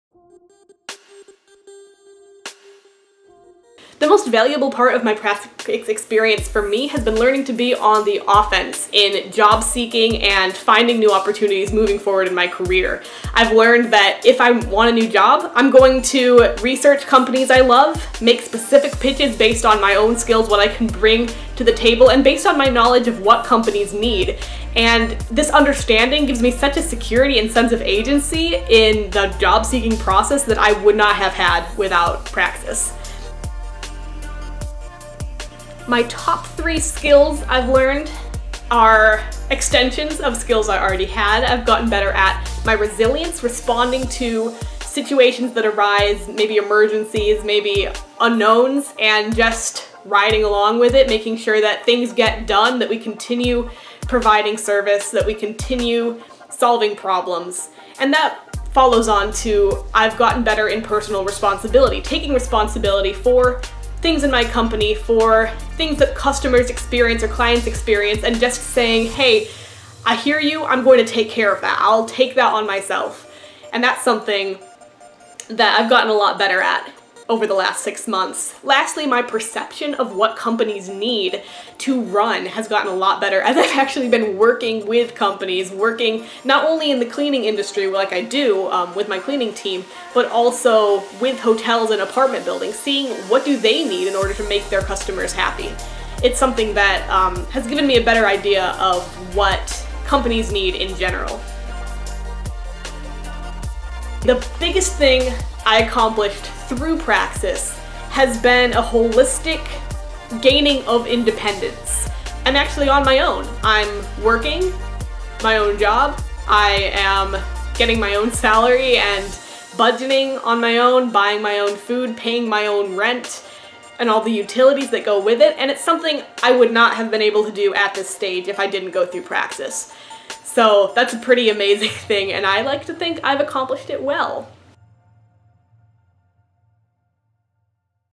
Meet Praxis Grad Emony Anderson
Emony Anderson graduated the Praxis program in August 2019. Here's what she has to say about the program.

Praxis is a twelve-month program that places young people with startups to apprentice, learn, grow, and walk away with valuable career skills.